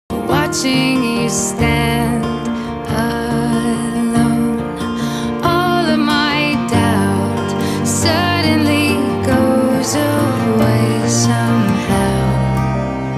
Cute Girls Pole Vault | Maria Roberta Gherca#shorts#shortsfeed#athletes#motivation#sports
Maria Roberta Gherca is a famous Italian athlete, pole vaulter, social media influencer, and entrepreneur from Genzano di Roma, Italy. She was born on March 9, 2000, and has participated in various national and international pole vault competitions. She has also a large following on Instagram and other platforms.